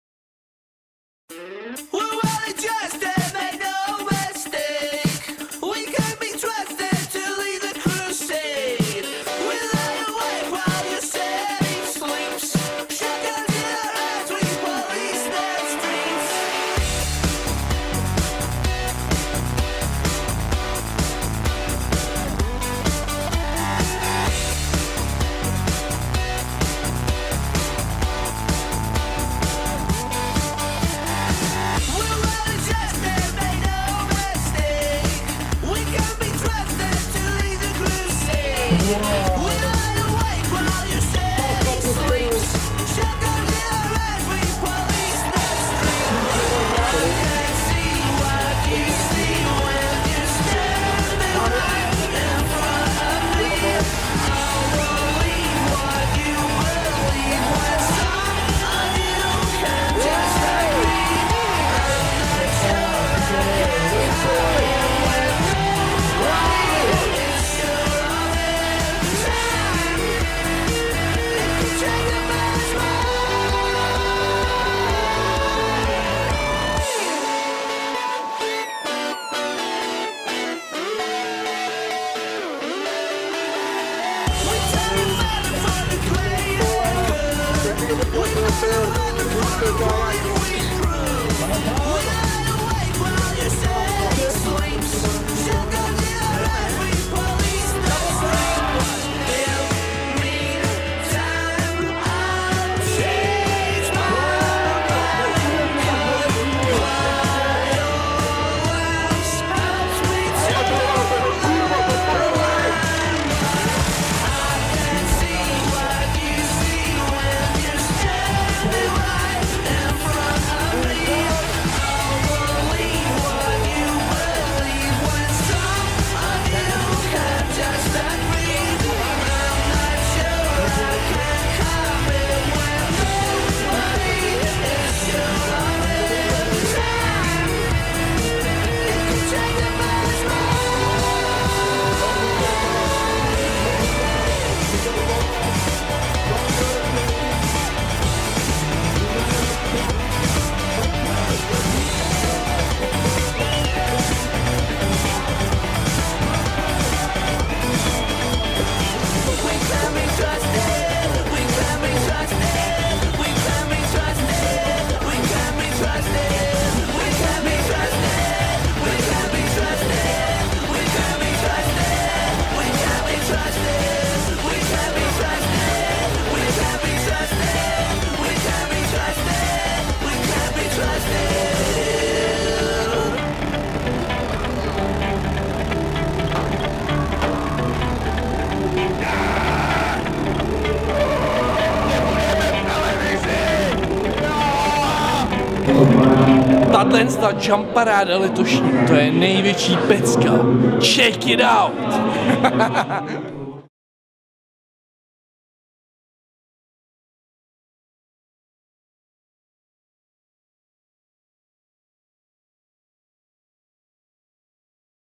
JumParade 2012
Video z třetího ročníku bikových zavodů ve Starém Plzenci